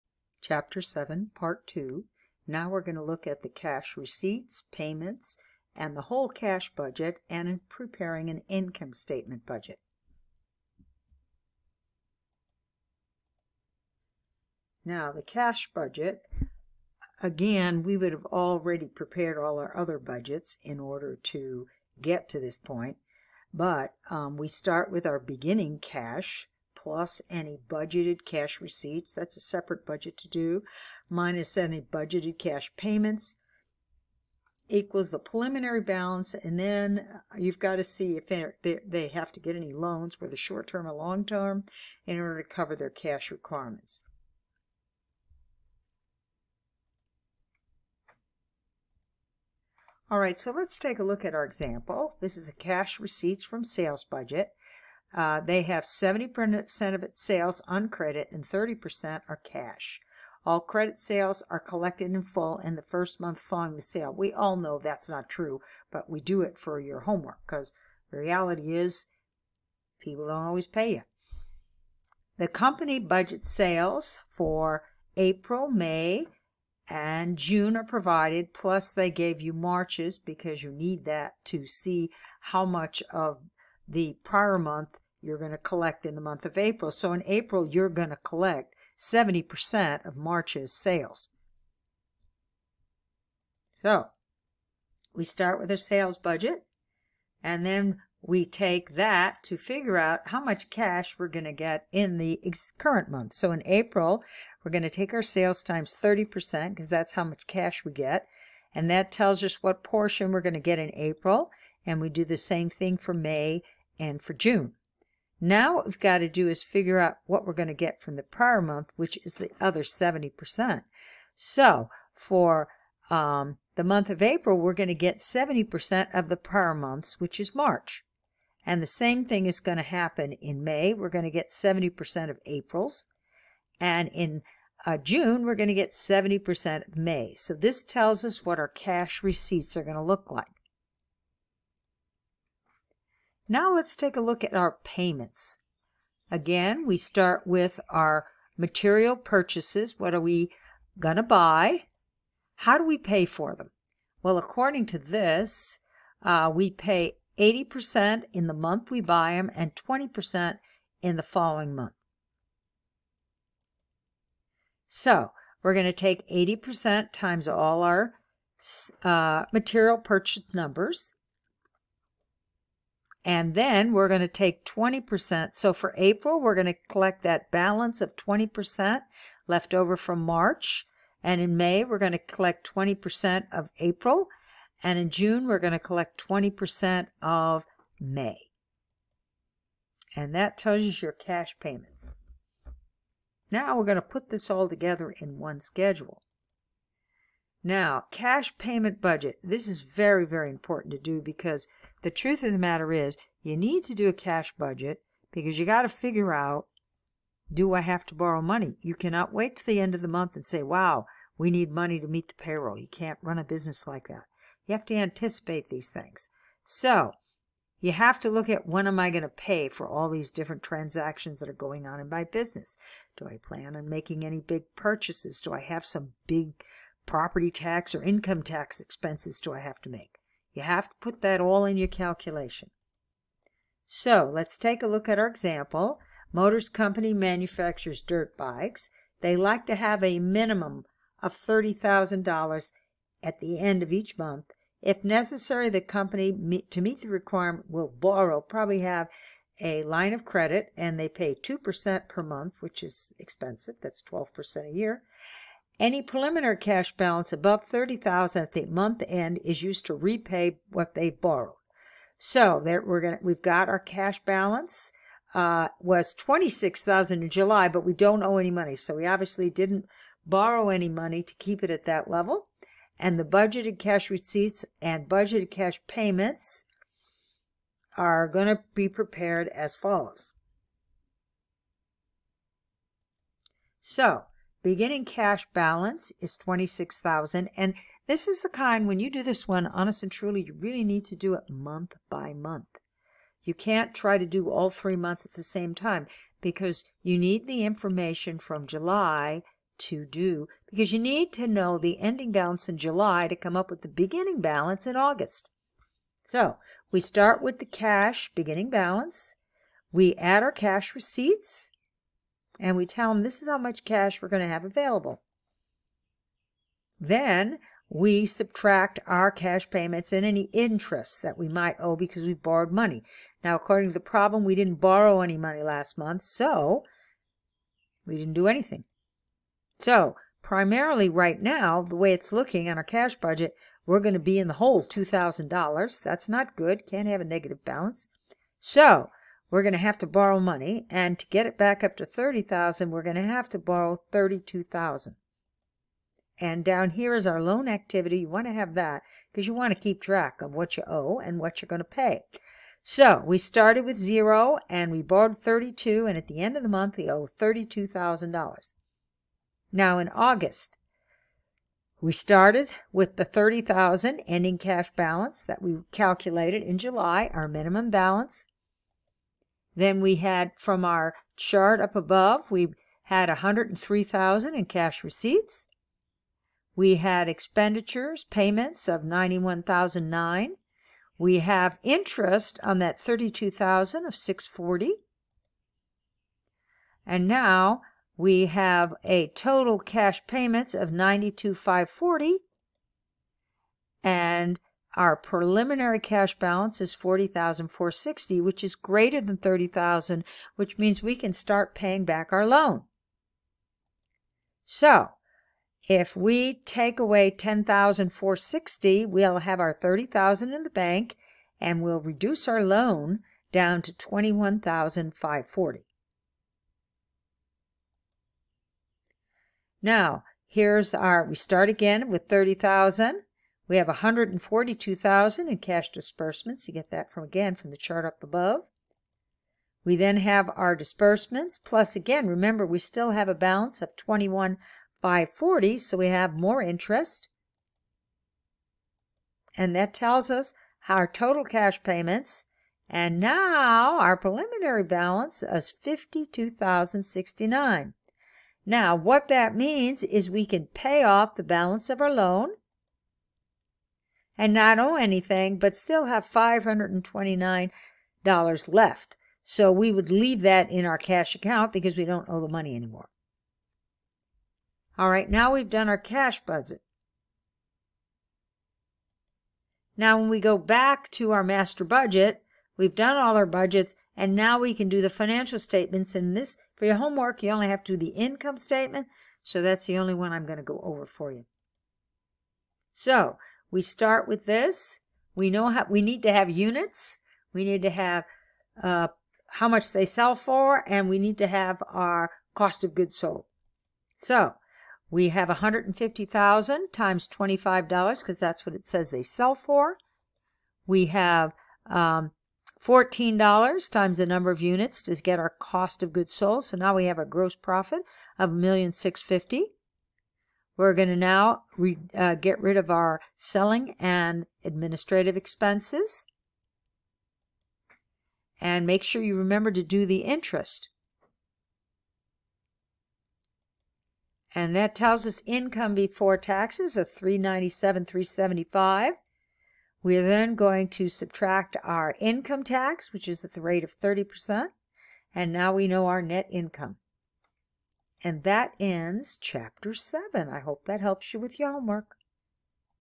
Chapter 7 Part 2 cash income statement budget
How to prepare a cash and income statement budget